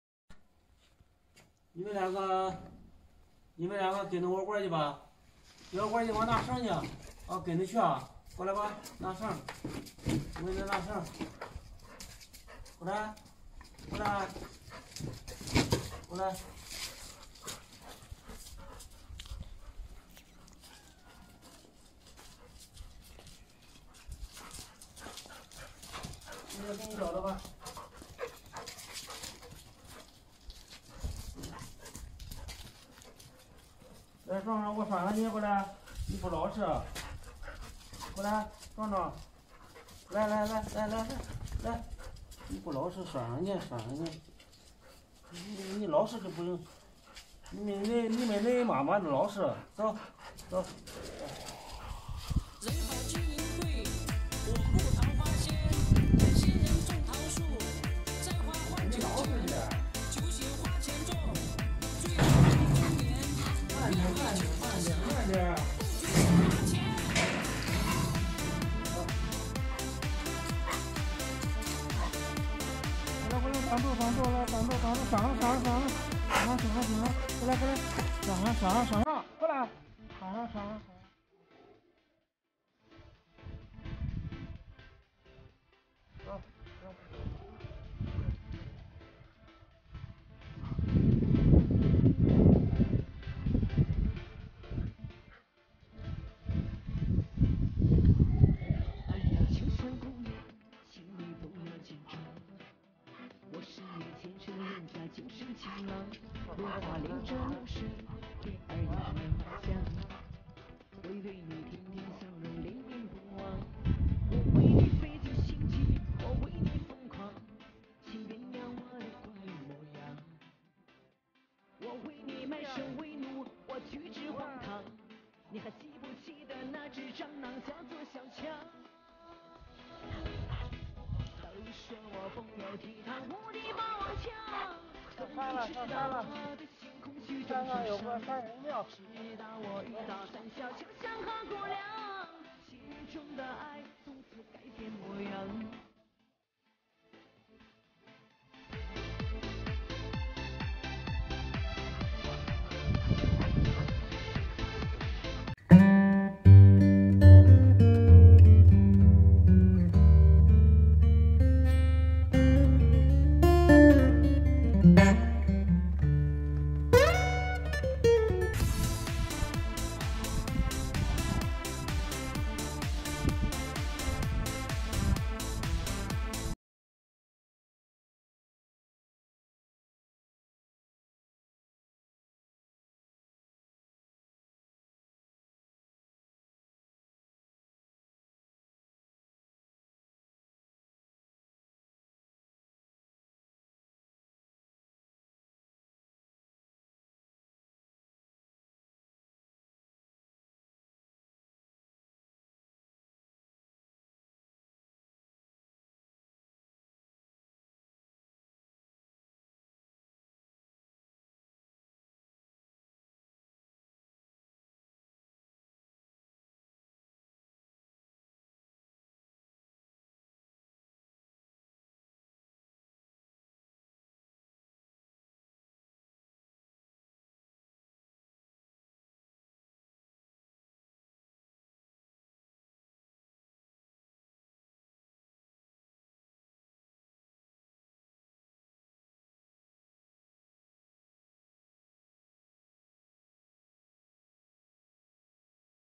爺爺帶毛孩子去爬山，老人放開壯壯牽引繩那叫一個瘋狂，玩嗨了【我是趙姐】
我们强调了视频内容的合规性，绝对没有任何不良引导或危险行为。點擊進入趙姐的大家庭：
爺爺帶著兩隻金毛上山，壯壯一放開牽引繩那叫一個開心，叫都叫不回來！